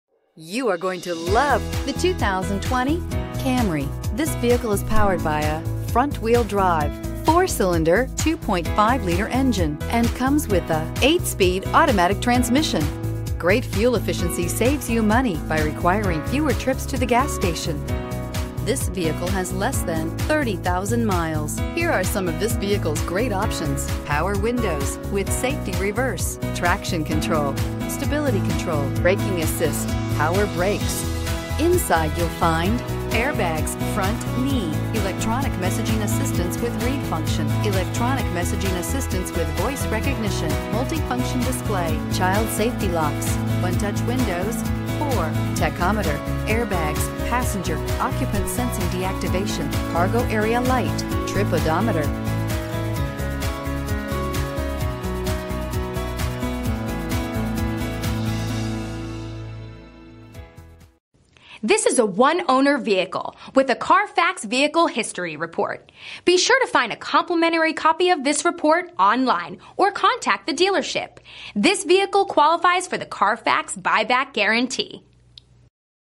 2020 Toyota Camry T46116A
2020 Toyota Camry LE

Cloth. Clean CARFAX. Blue 2020 Toyota Camry LE FWD 8-Speed Automatic 2.5L I4 DOHC 16V Recent Arrival!